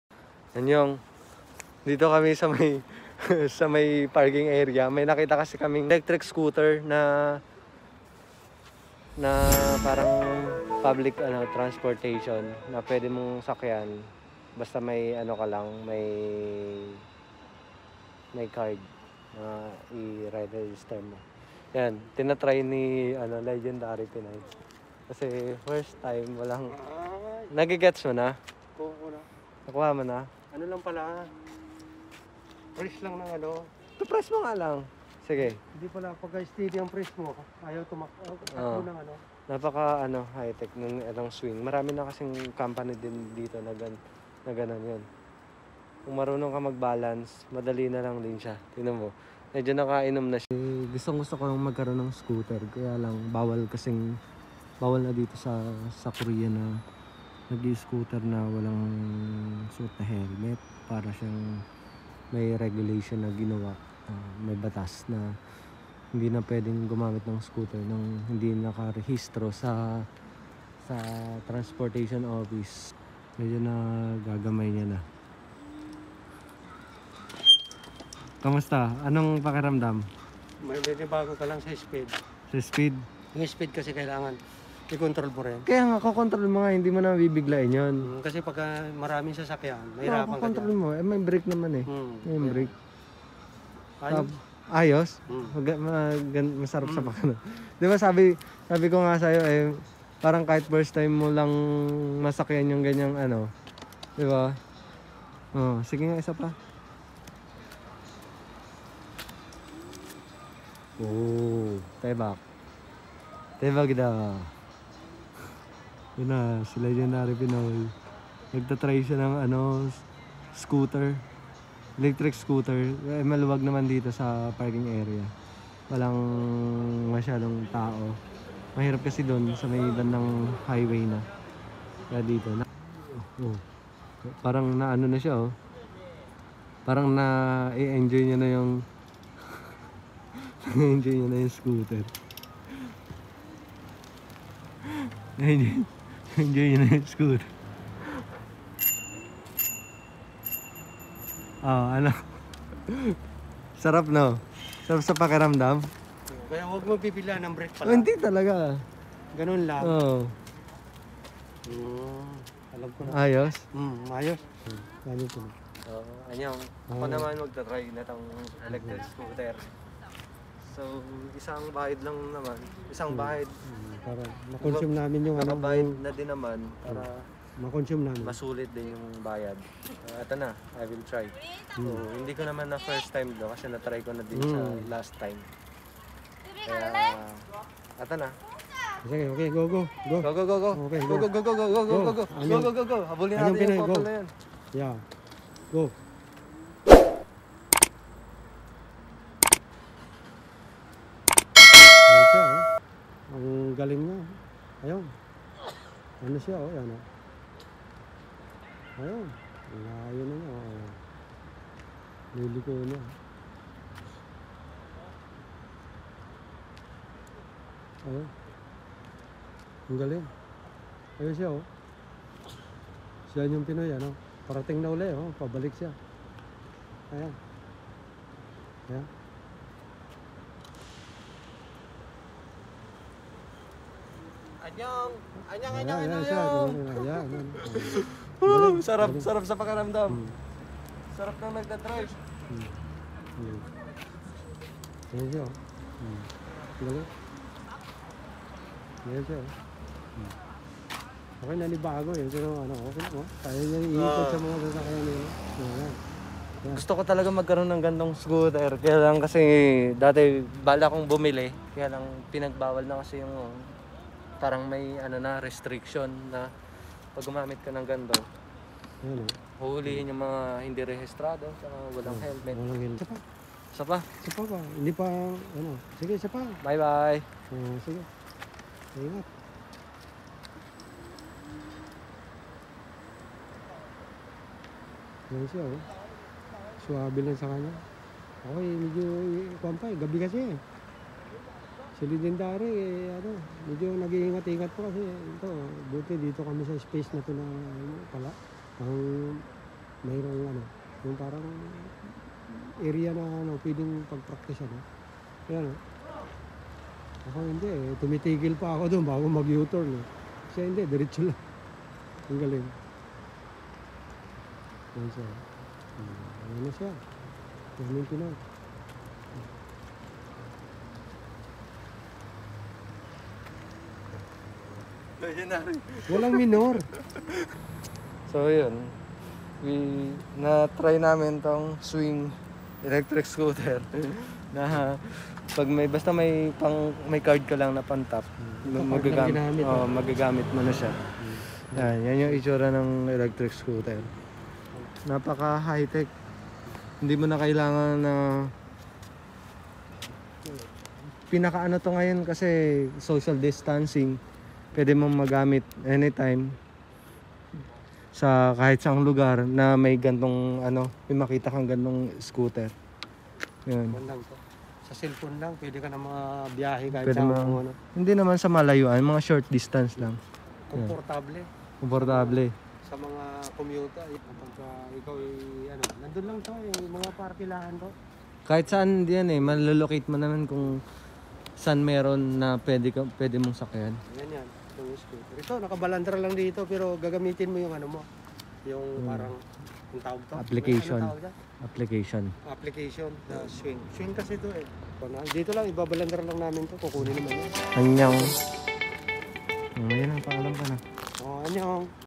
SARAP BUMALIK SA PAGKABATA.
first time namin subukang  sakyan ang electric kickboard or electric scooter